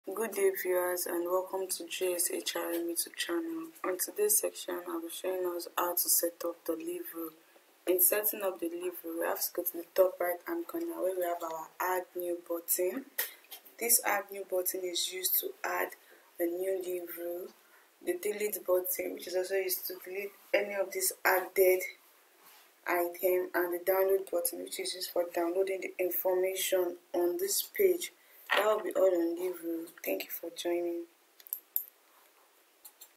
Leave rule setup
In this video, our consultant explains how to set up leave rule for an organisation on the GOSHRM application.